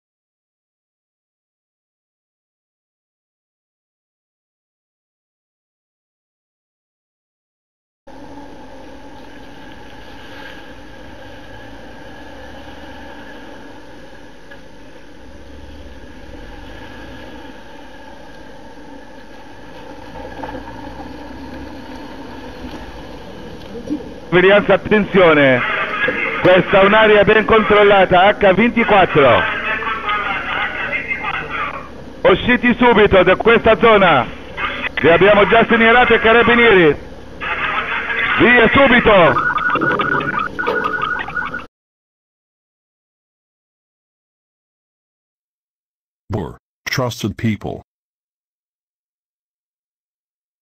BOR :Sventato tentativo furto in cantiere edile - Telepresenza e deterrenza h24
Tentativo di furto sventato in cantiere edile.
Il rapidissimo intervento del nostro operatore mette in fuga il malvivente sventando l'azione criminosa.
AFFIDATI A NOI!

Per contatti:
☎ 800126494

Ladri in casa - Furti in diretta - Furti sventati - Proprietari felici.